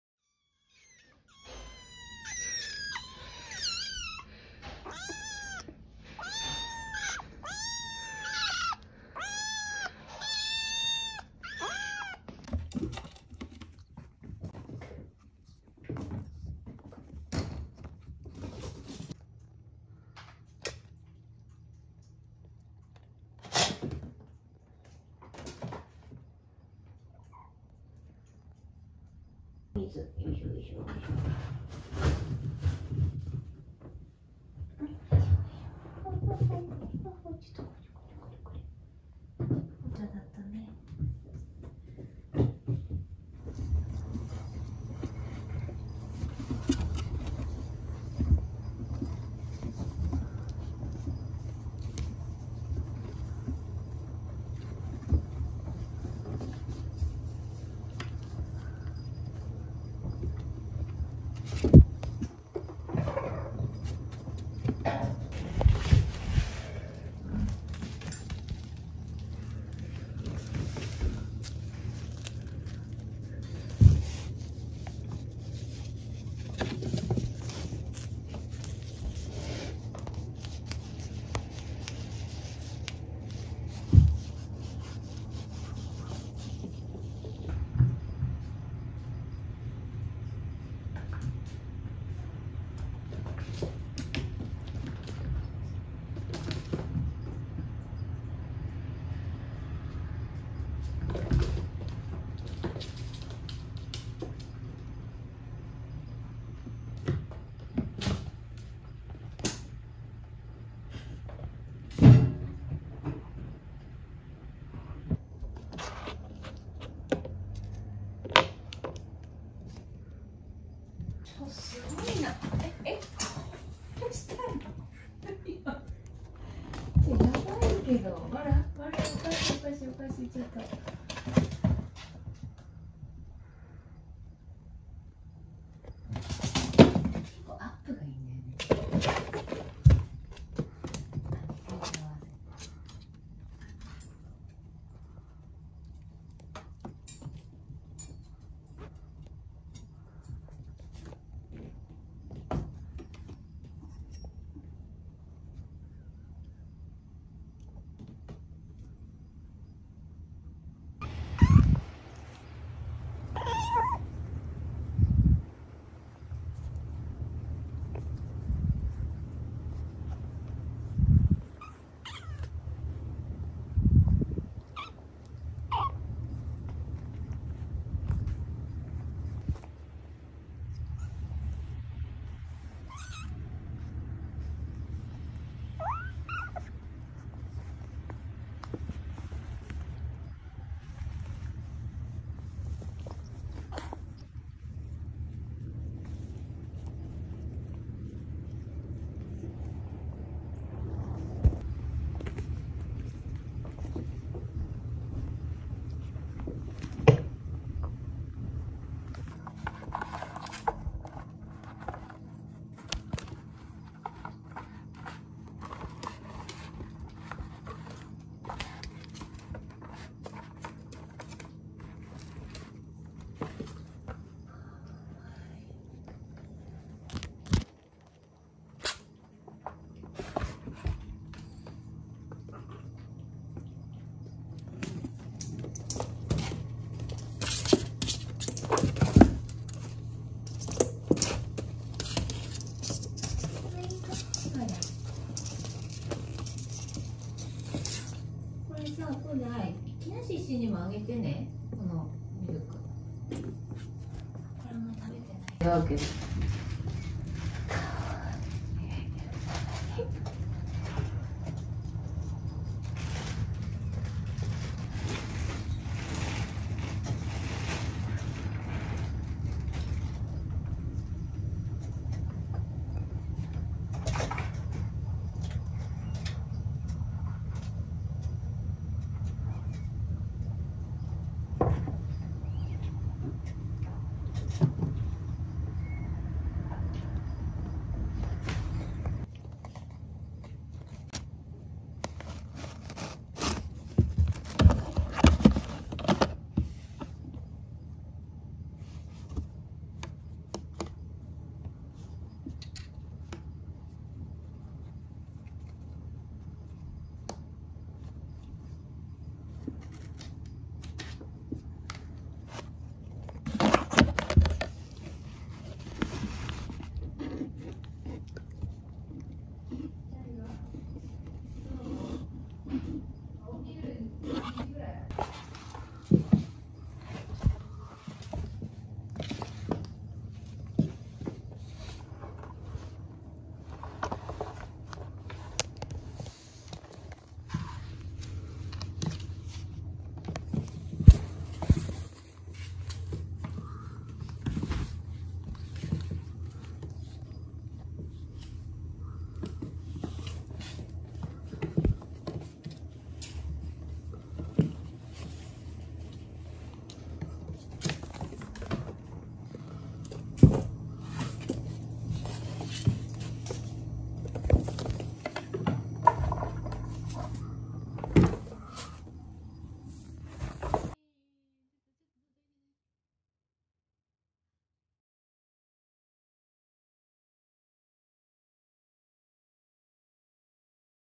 Two Kittens Playing Non-Stop 🐱💕 48 Days Old and Loving the Air Gun Craze
Our story starts from the rescue of two tiny, one-day-old abandoned kittens weighing just 57g and 75g.

Timestamps:
00:00 Hungry Chorus
00:13 PlayingTime Begins
00:44 Exploring Inside the Desk
01:06 KINAKO and WARABI Playing with Toys